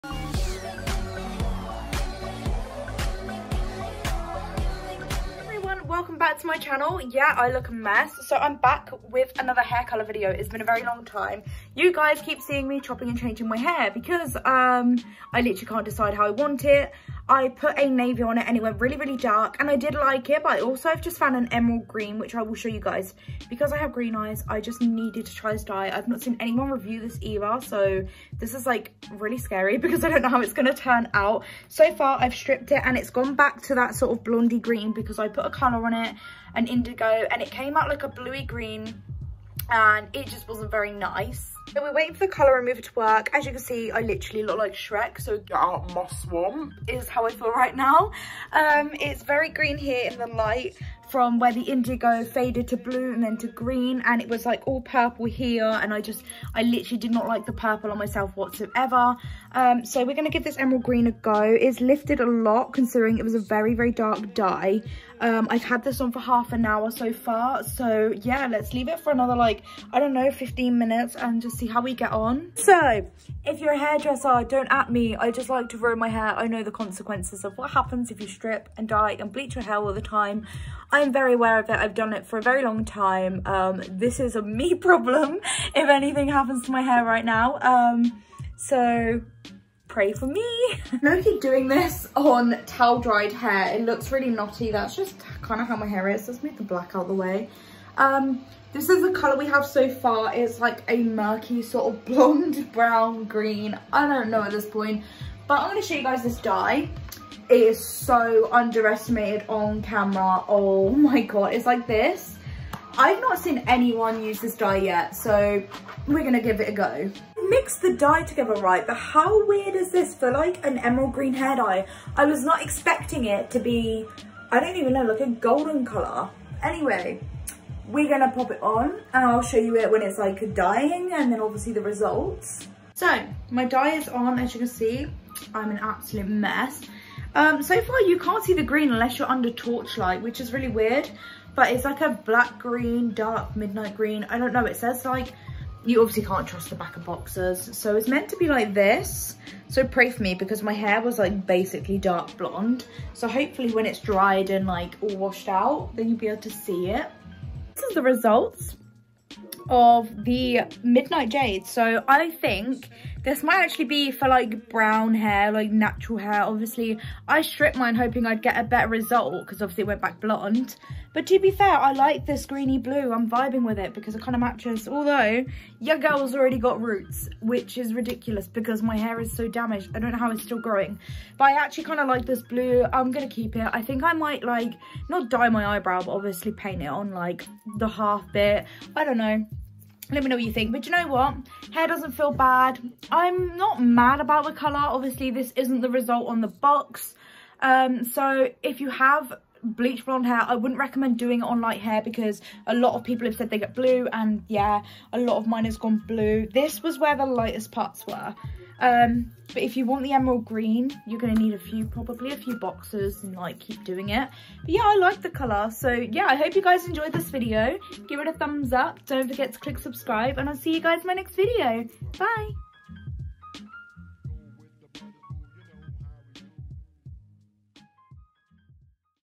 I DYED MY HAIR BLACK TO GREEN?! | LIVE MIDNIGHT JADE HAIR DYE
Hello everyone! So I’m back with another hair dying video! I’m sorry to any hair dressers watching right now! I hope you enjoyed this little